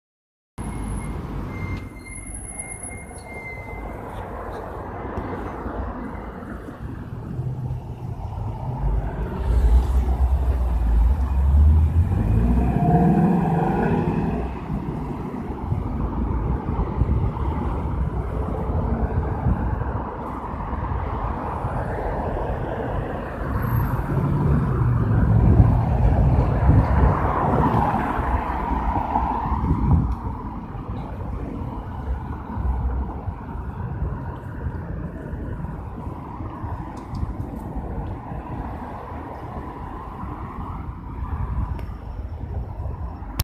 Ford Mustang GT 350 Shelby Construction Zone Acc
White Ford Mustang GT 350 Shelby construction site flyby.